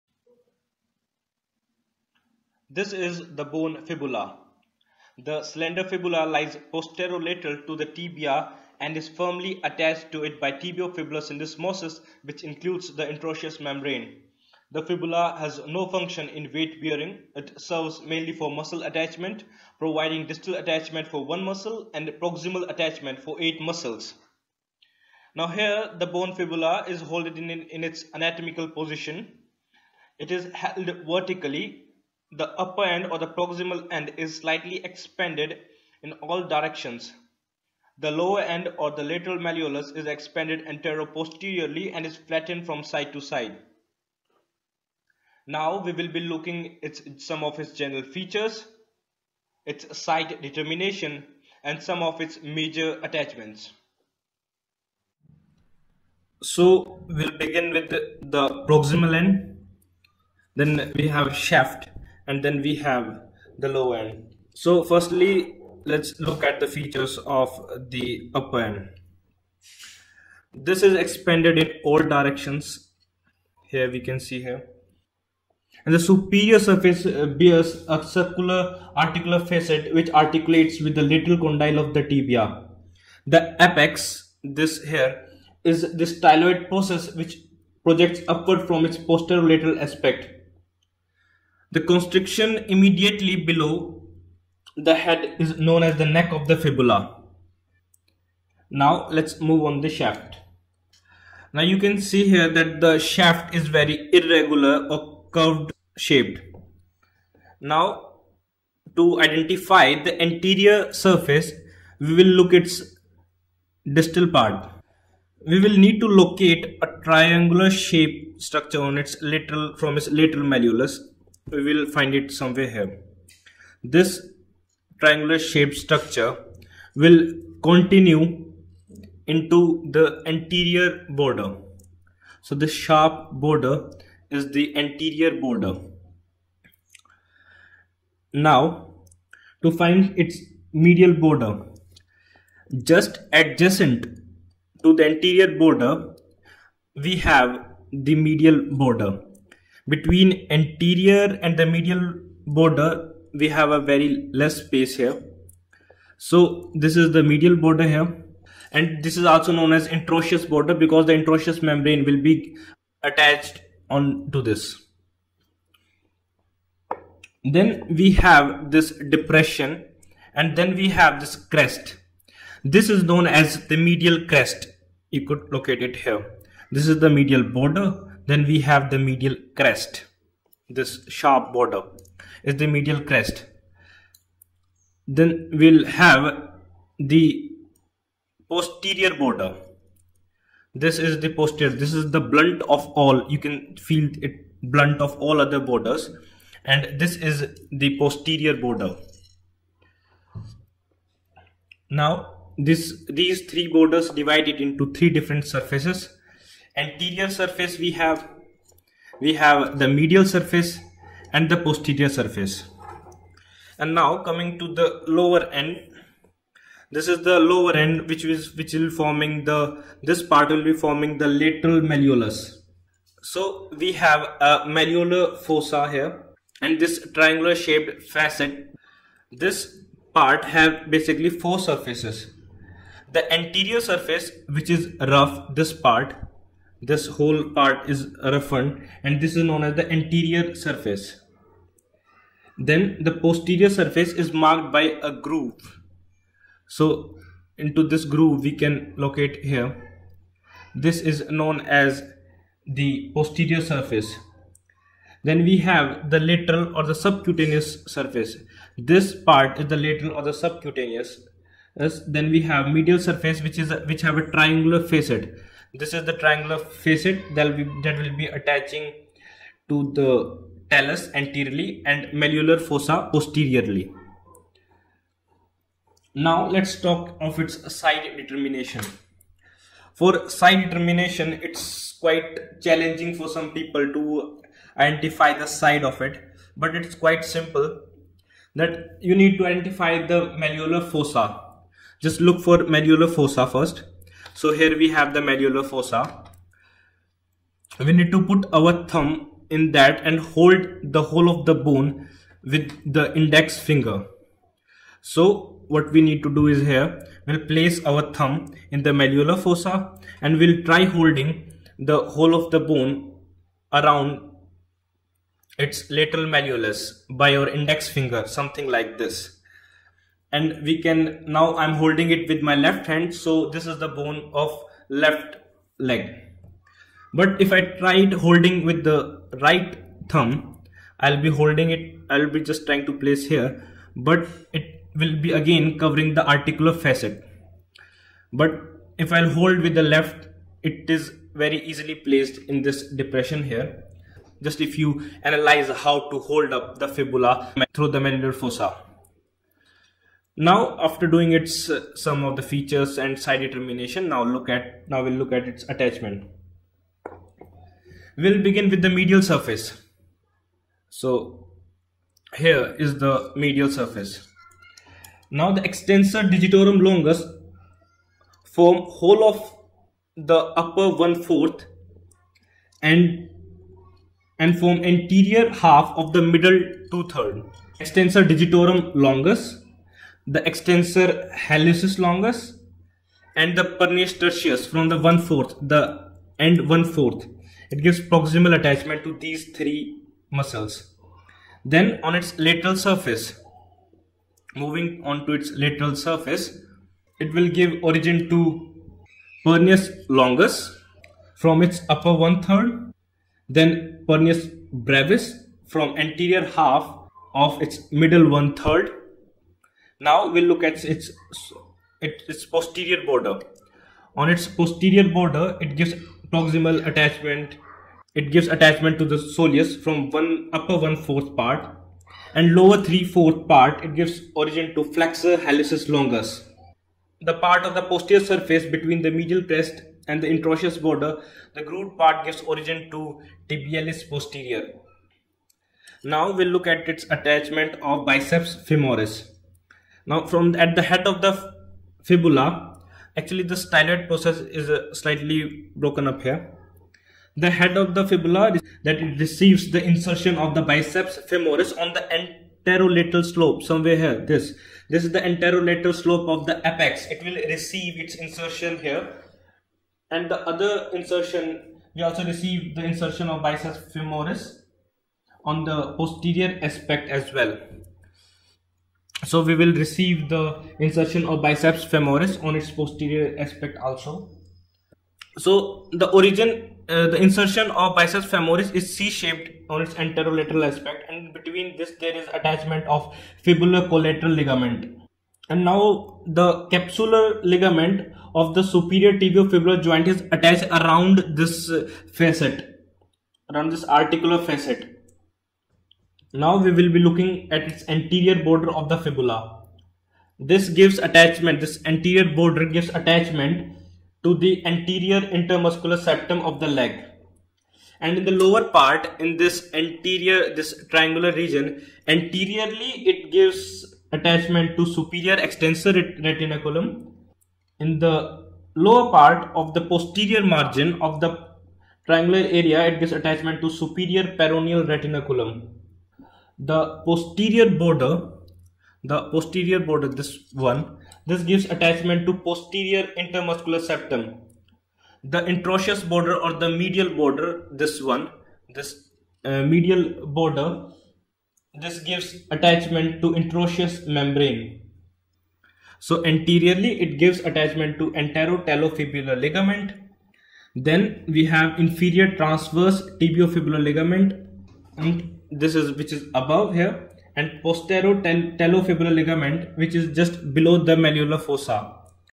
Fibula Bone Features, side determination, and attachments anatomy explained | MBBS | VIVA
Fibula Bone Features, side determination, and attachments simplified for quick revision.
Structures identified:
Head
Lateral malleolus